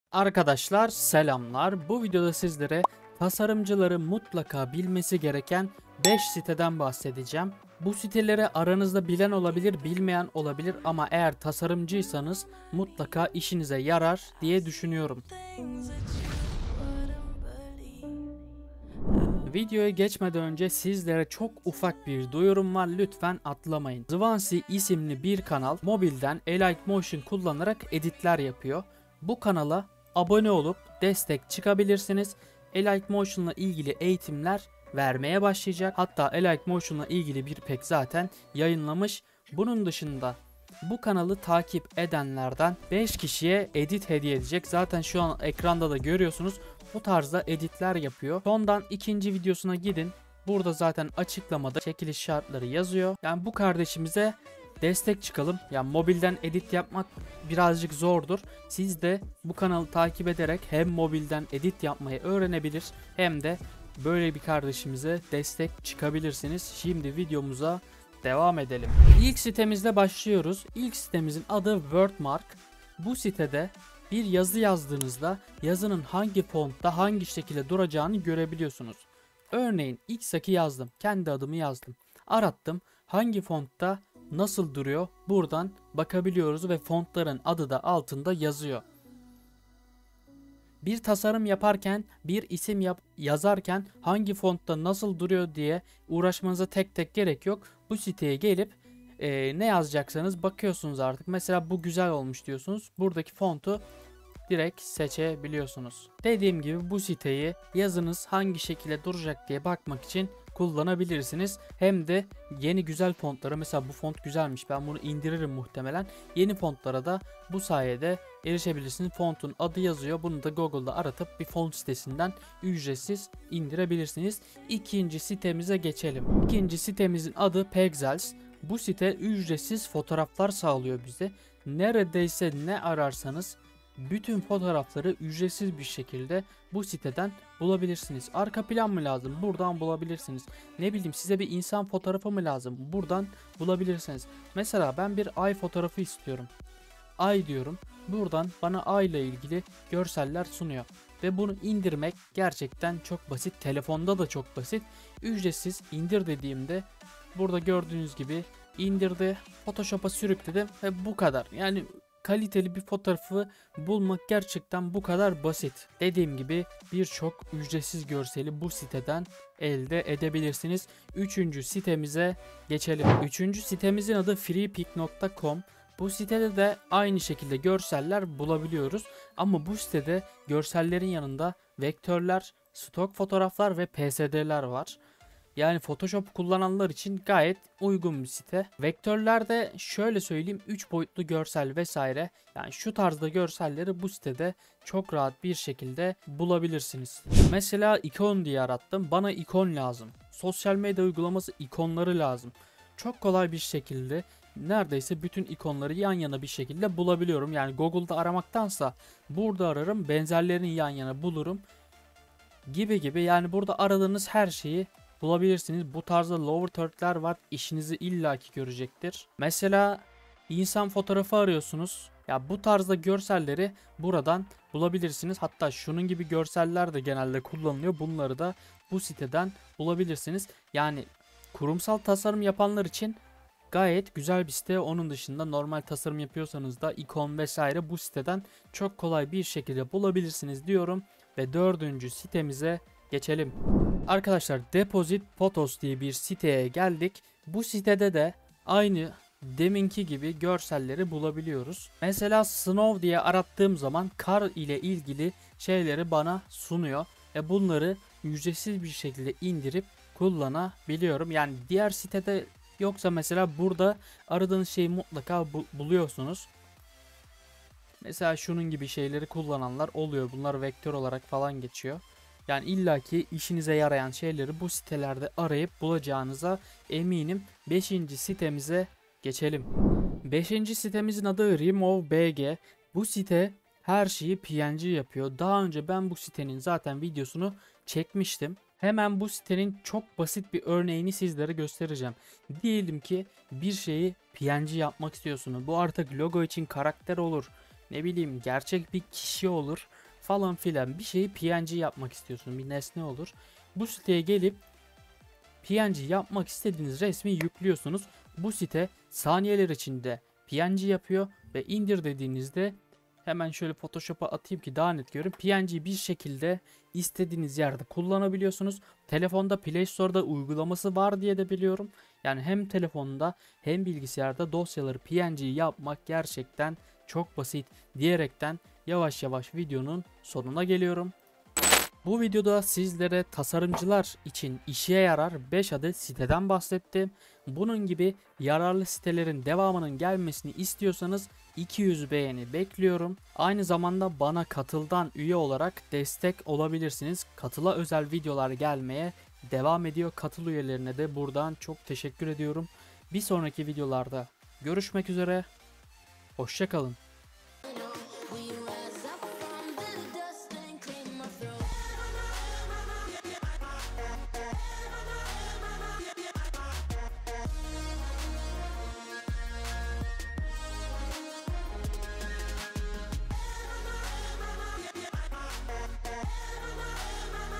Tasarımcıların Mutlaka Bilmesi Gereken 5 Site !
Selam arkadaşlar bu videoda Tasarımcıların Mutlaka Bilmesi Gereken 5 Siteden bahsettik iyi seyirler.

Video içeriği
00:00 Giriş
00:19 Reklam
01:08 1. SİTE
02:06 2. SİTE
02:53 3. SİTE
04:04 4. SİTE
04:40 5. SİTE